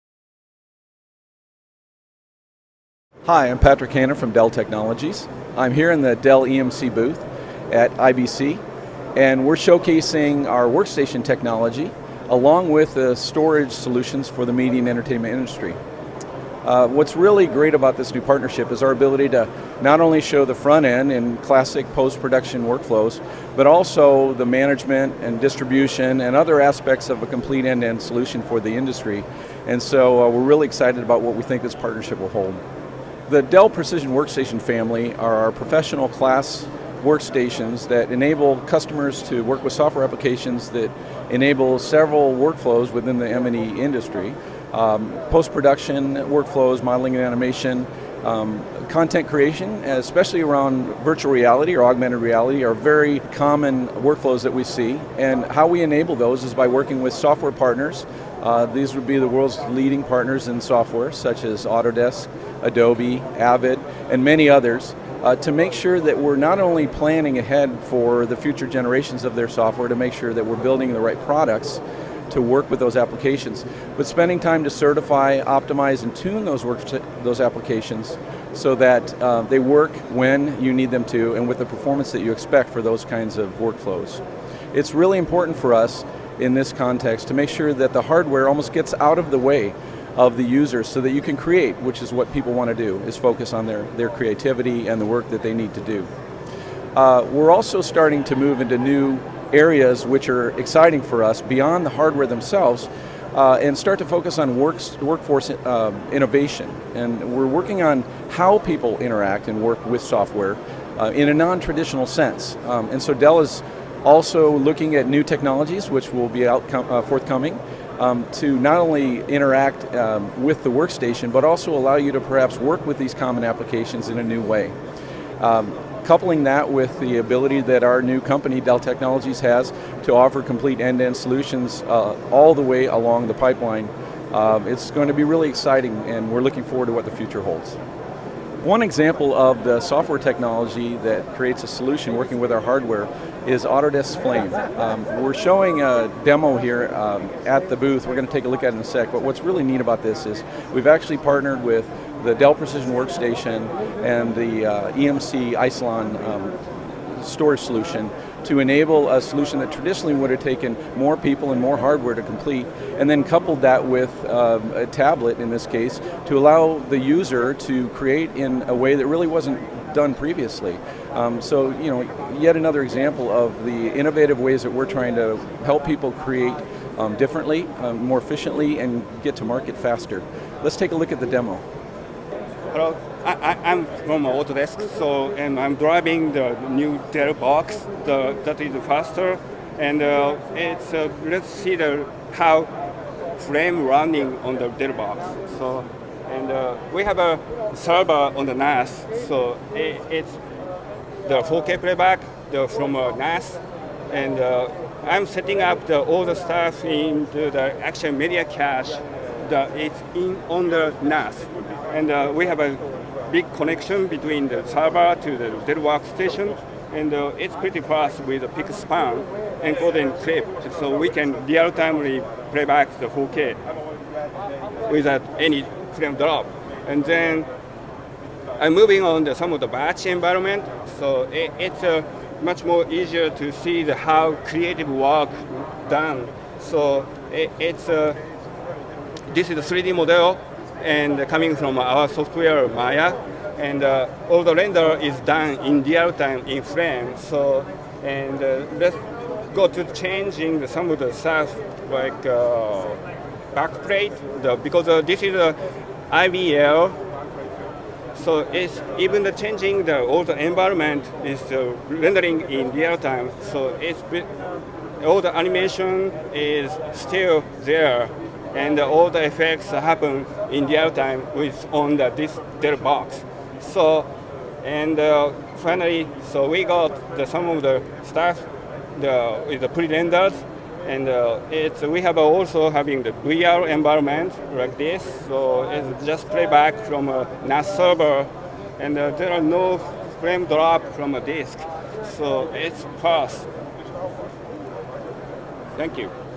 IBC 2016: Dell Technologies' Patrick Hannan
Dell's Patrick Hannan talks about the company's workstation technology as well as EMC's storage solutions for media and entertainment. The two companies are now one.